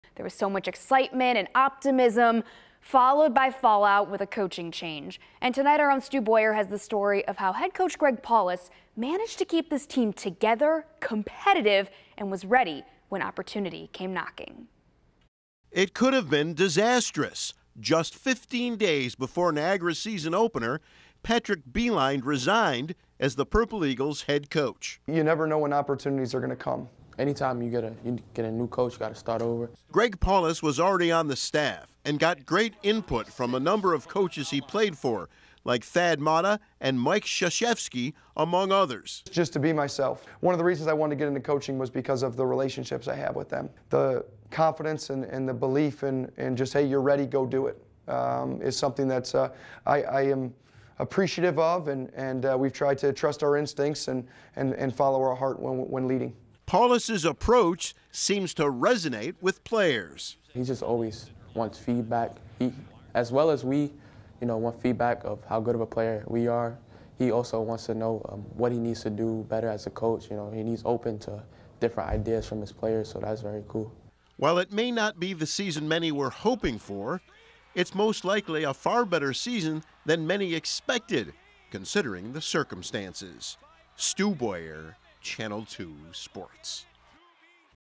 Greg Paulus ready when opportunity knocked
Paulus's approach seems to resonate with players.   As well as we want feed back how good of a player we are he also wants to know what he needs to do better.